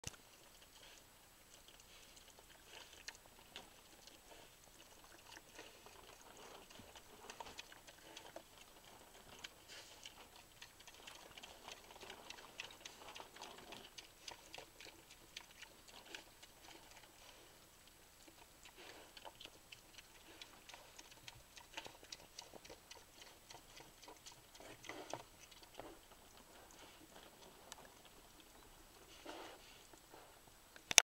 piglet2
Our Mini-Pigs - Part 2
Our little mini-pigs were born 2 years ago and have grown quite a lot since. So maybe I can make a little video to show them off!